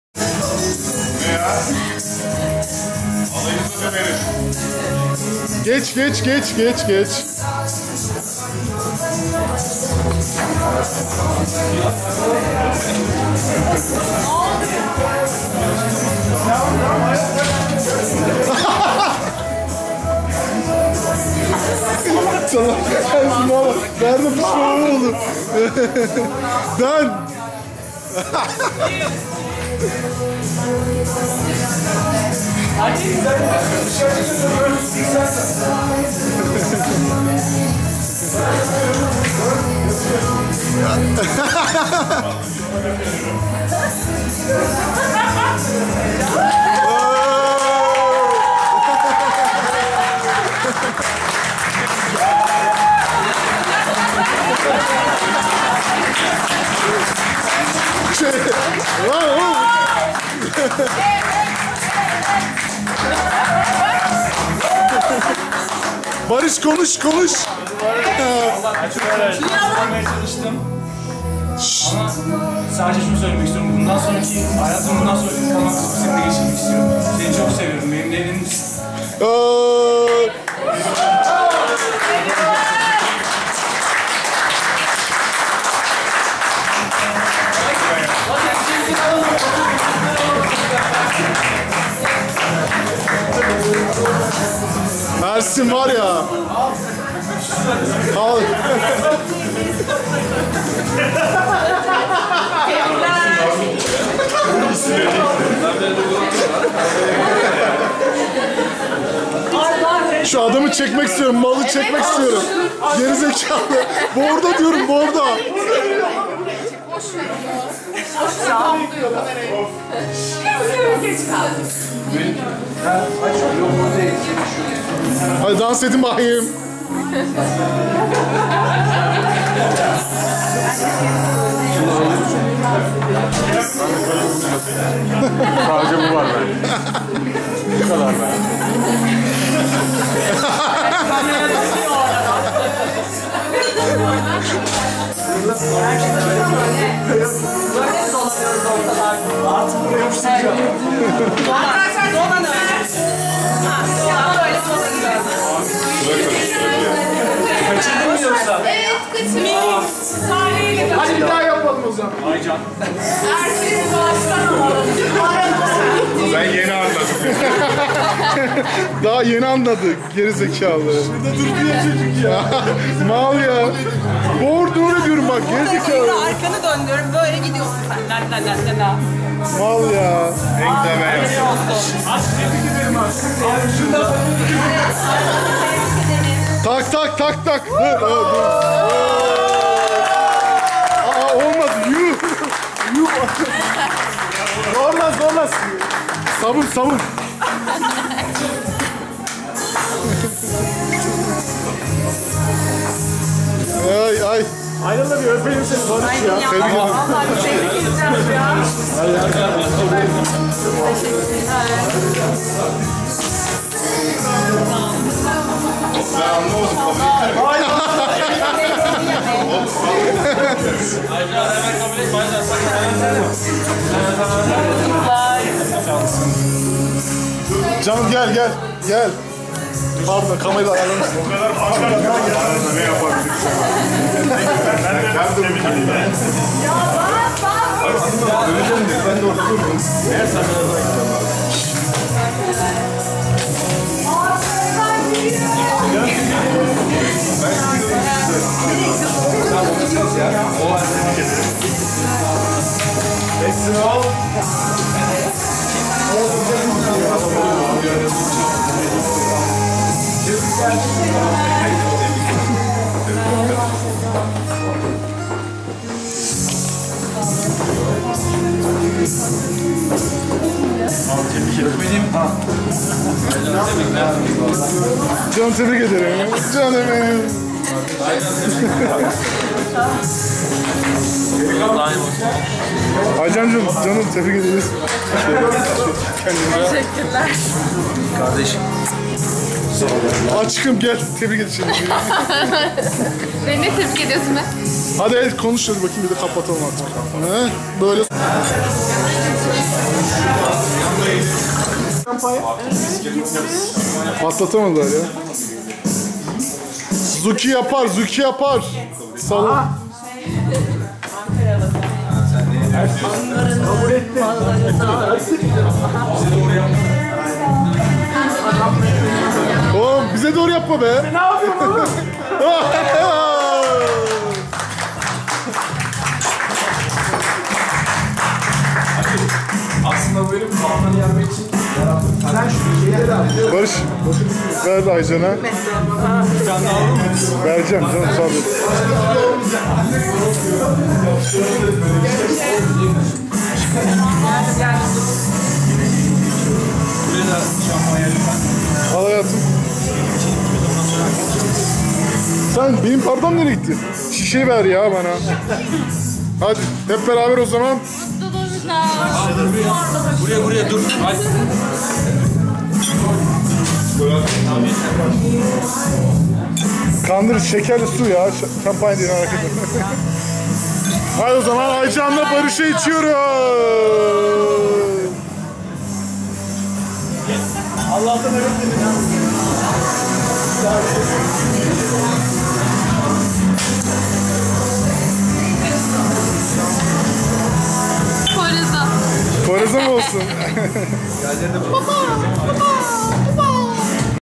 Aycan & Barış - Evlilik Teklifi
Barış'ın Aycan'a yaptığı süpriz evlilik teklifini sizin için kameraya aldık :) Tabiki gülmek için :P